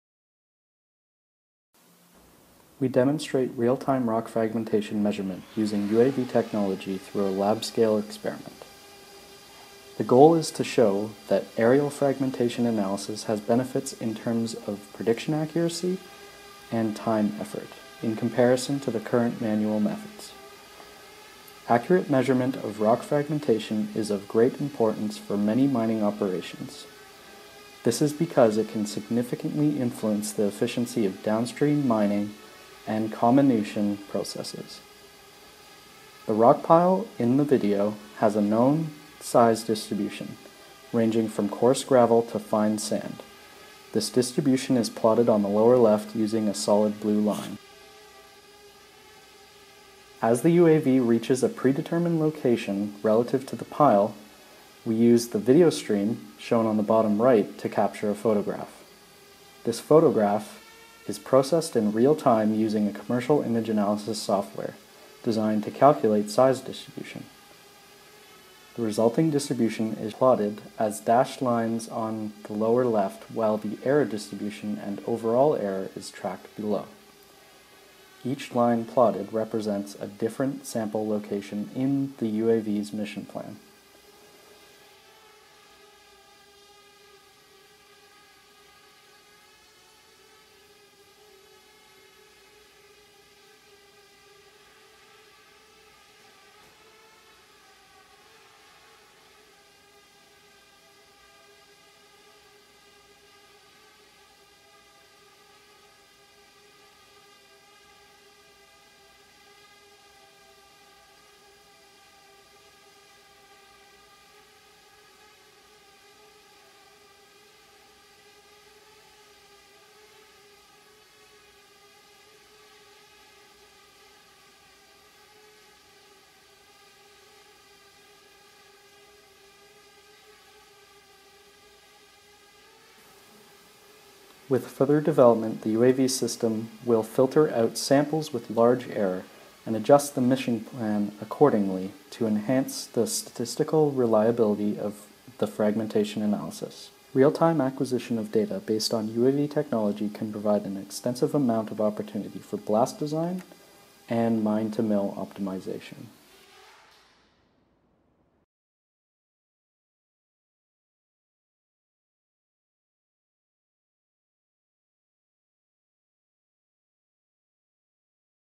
Innovations in Mining: Real-time Fragmentation Analysis Using UAV Technology
In this video, we show preliminary results of using UAVs for rock fragmentation analysis.

Work by  Thomas Bamford,  Prof. Kamran Esmaeili (University of Toronto, Civil Engineering), and Prof. Angela P. Schoellig (University of Toronto, Institute for Aerospace Studies).

The research was conducted at the Dynamic Systems Lab University of Toronto Institute for Aerospace Studies (UTIAS), Canada.